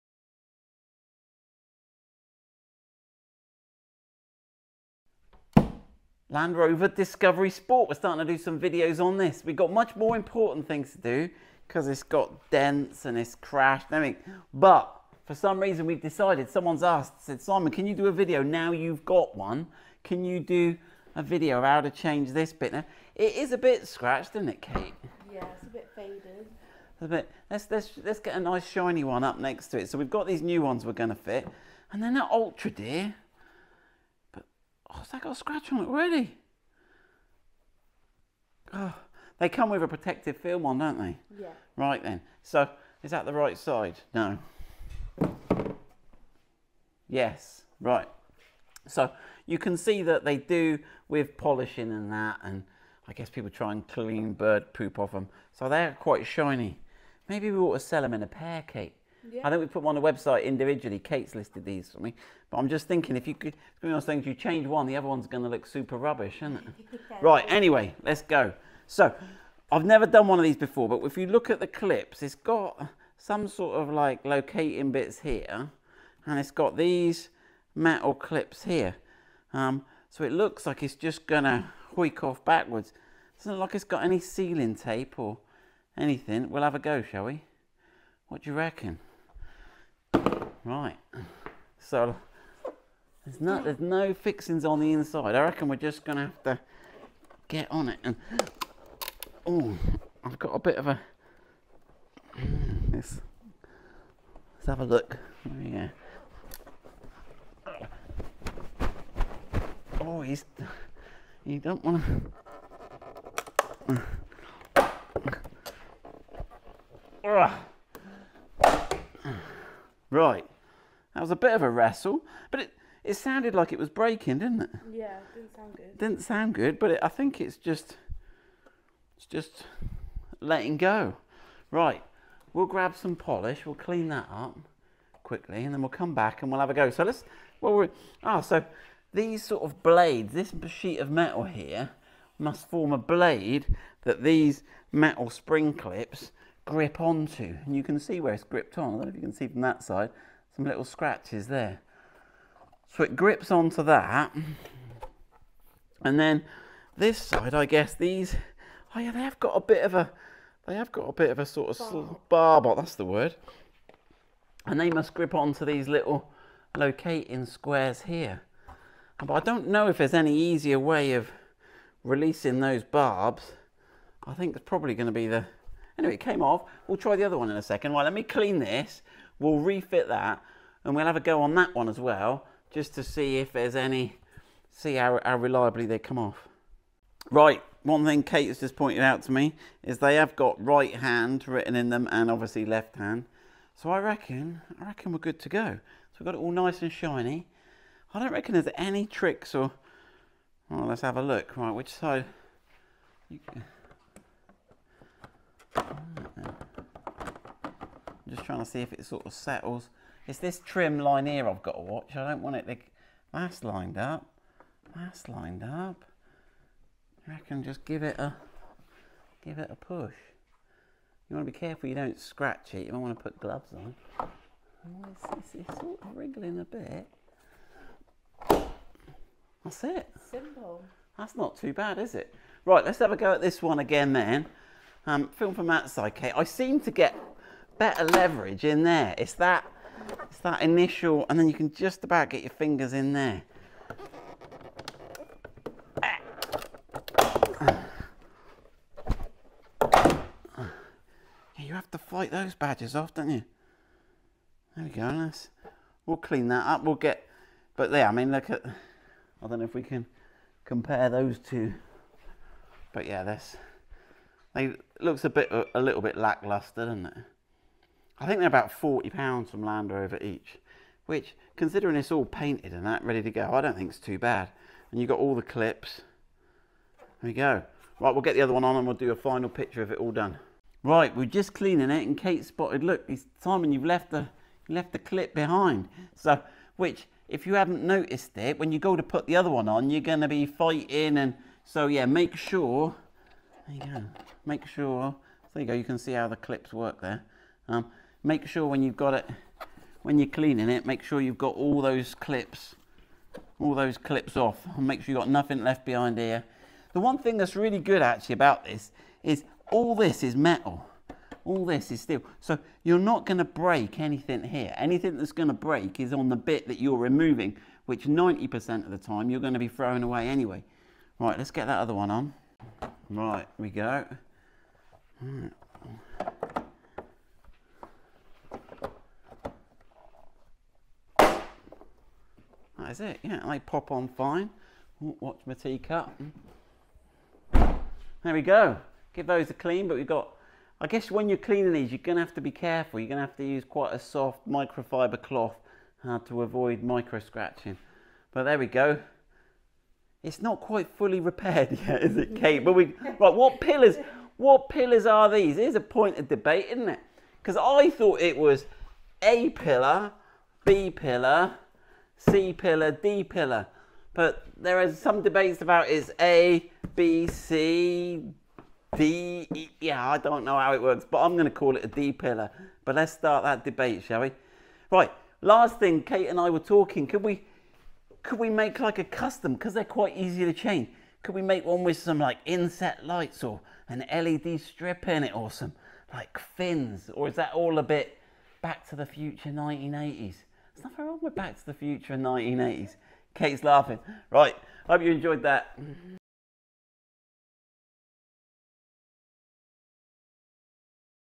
How to Remove Land Rover Discovery Sport L550 "D" Pillar Cover Trim
join us as we replace the gloss black rear D Pillar trims on our Land Rover Discovery Sport L550.